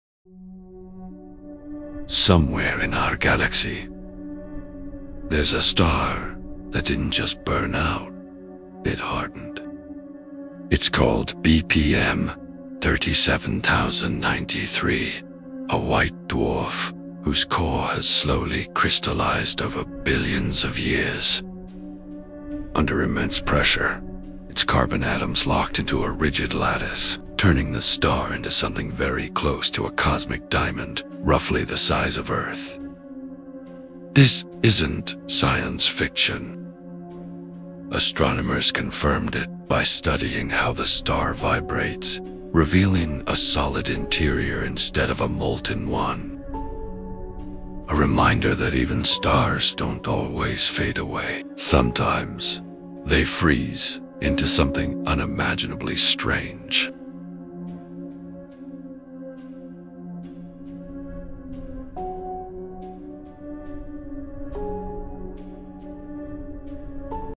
BPM 37093: The Diamond Star💎
Deep in our galaxy lies BPM 37093,
a white dwarf unlike any other.

As this star cooled,
its carbon core didn’t remain plasma
it crystallized.

Astronomers confirmed this using
stellar vibration data
showing that nearly 90% of the star’s mass
has frozen into a solid carbon lattice.

BPM 37093 is essentially
a planet sized diamond
billions of trillions of tons of carbon
locked in crystal form.

It’s not science fiction.
It’s stellar evolution
written in stone.